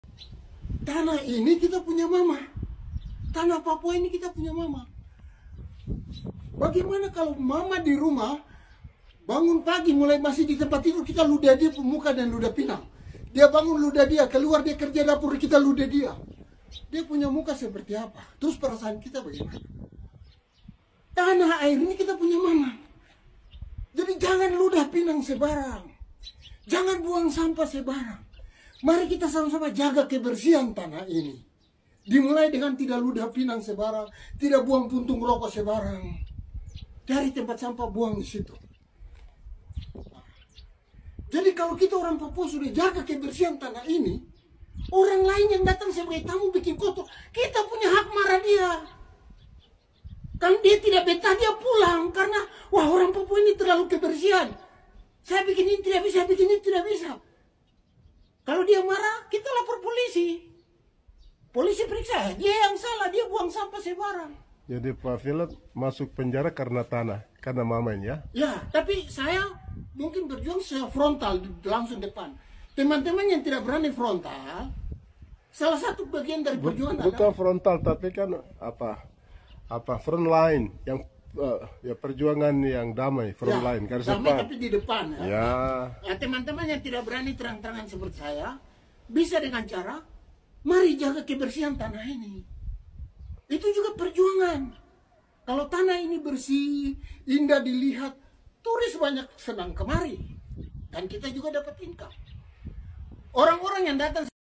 Tanah adalah "Mama" Bpk Filep Karma!Tapol Papua -wenda channel
Tanah adalah mama, kita meludah pinag ke tanah itu sama dengan kita sedang ludah kita pu mama, jadi stop ludah pinang sembarang!